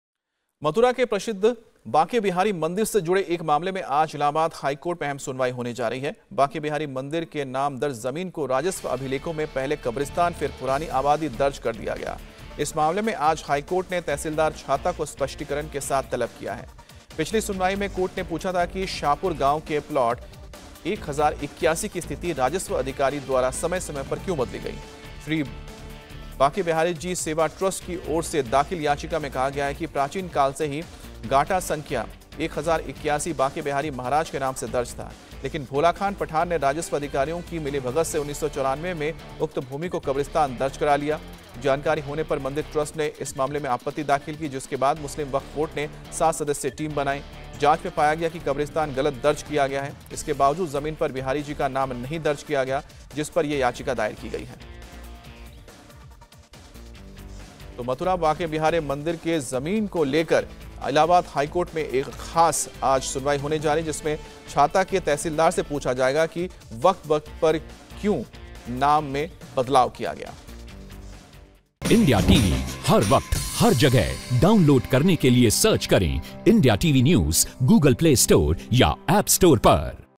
Banke Bihari Temple: मथुरा के बांके बिहारी मंदिर की जमीन को मिली भगत से कब्रिस्तान दर्ज कराया गया ?
Banke Bihari Temple: बांके बिहारी मंदिर नाम दर्ज जमीन को राजस्व अभिलेखों में पहले कब्रिस्तान फिर पुरानी आबादी दर्ज कर दिया गया...  इस मामले में आज हाईकोर्ट ने तहसीलदार छाता को स्पष्टीकरण के साथ तलब किया है.. पिछली सुनवाई में कोर्ट ने पूछा था कि शाहपुर गांव के प्लाट 1081 की स्थिति राजस्व अधिकारी द्वारा समय-समय पर क्यों बदली गई..